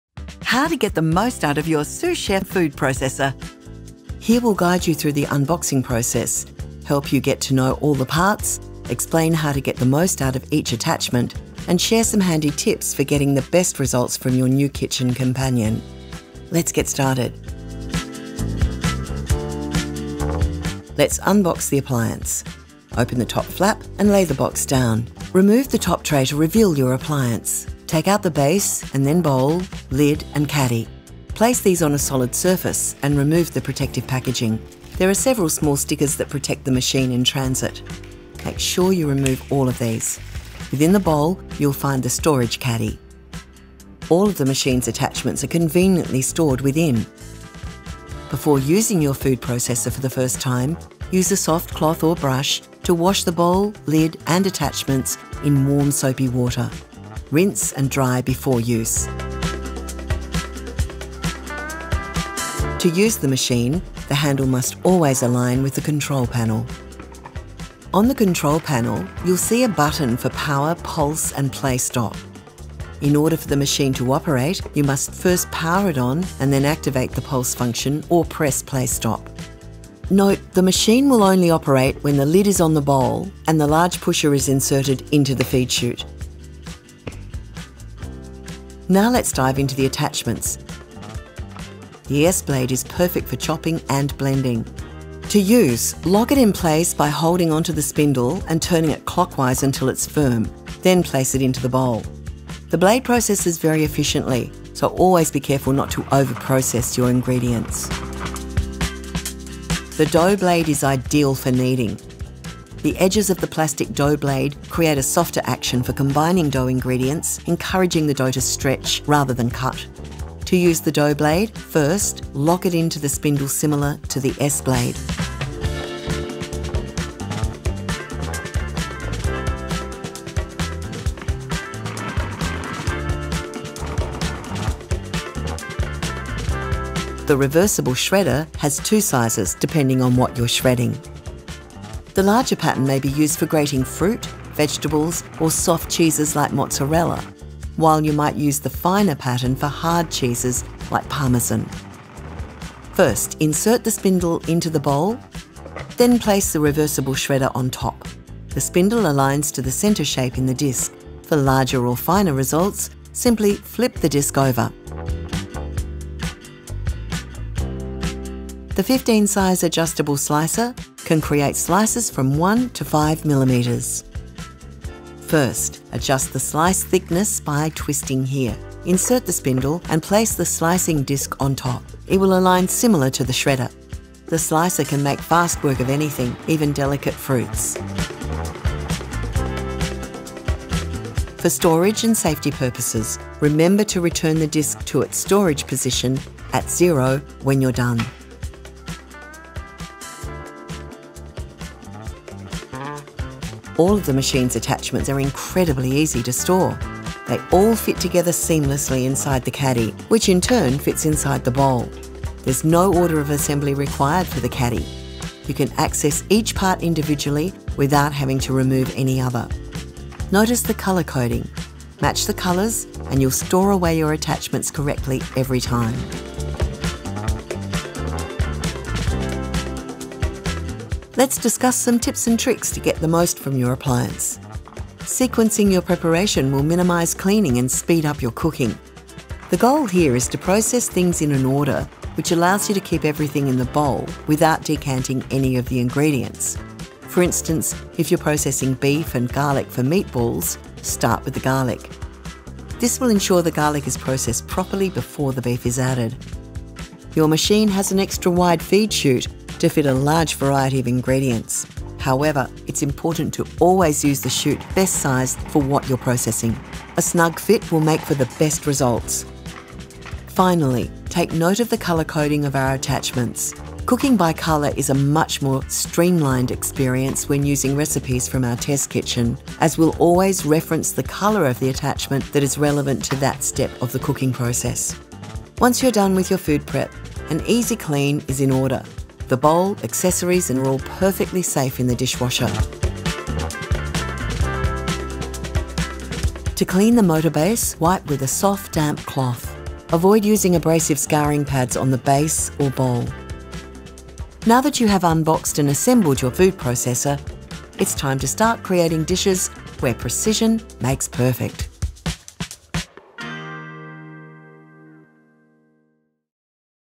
the Breville Sous Chef® 9 | Find out how to get the most out of your food processor | Breville USA
Discover how to unbox, use, and clean your new Breville Sous Chef® 9 base, bowl, lid and caddy.

Here’s everything you need to know before using your Breville Sous Chef® 9 food processor, from how to remove the protective packaging to how to clean your appliance. Discover our guide to the caddy of Breville Sous Chef® 9 attachments, including how to use, store and clean each attachment. Topped off with our general Breville Sous Chef® 9 tips and tricks, you’ll be slicing, dicing and shredding your way to delicious dishes in no time.

00:00 How to unbox the Breville Sous Chef® 9 food processor
01:02 the Breville Sous Chef® 9 attachments and features explained
03:22 What’s included in the Breville Sous Chef® 9 caddy?
03:48 the Breville Sous Chef® 9 tips and tricks
04:45 How should you clean your Breville Sous Chef® 9?